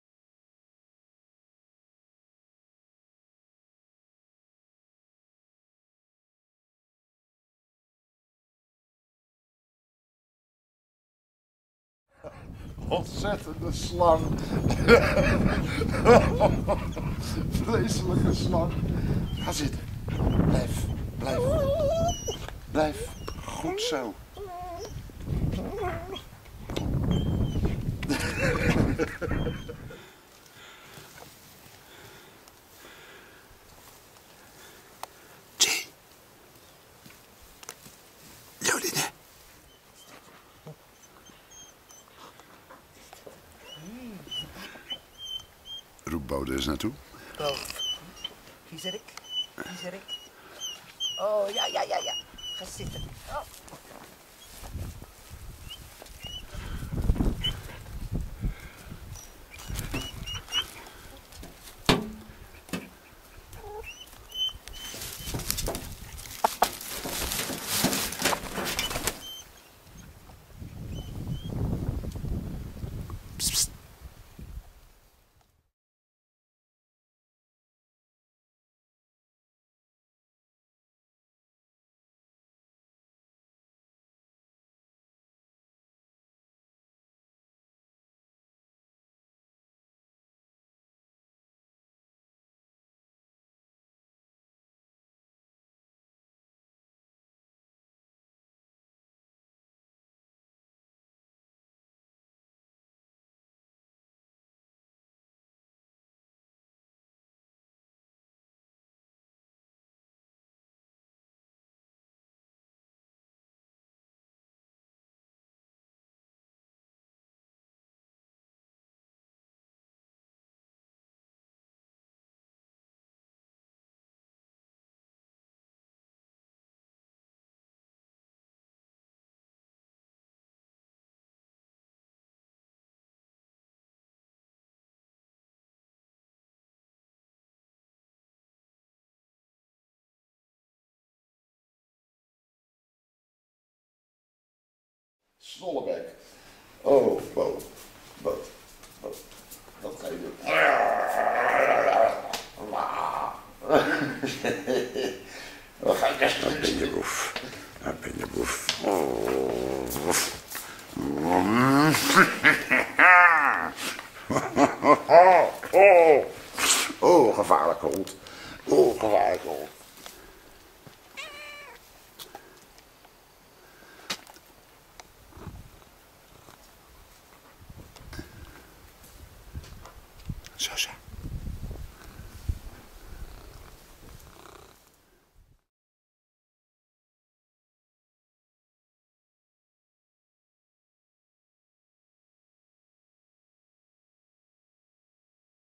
Beau the Movie
Onze nieuwste aanwinst. een zgn. Tricolor Border Collie. De laatste eigenaar had geen click met deze hond en had dus besloten dat hij beter maar in het asiel kon worden gedumpt, met als consequentie het gashok. Daar hebben wij dus een stokje voor gestoken en dus hebben wij nu een kanjer van een border erbij die graag, heel graag wil leren. Dit is een korte samenvatting van hoe hij er na twee weken reeds bijstaat.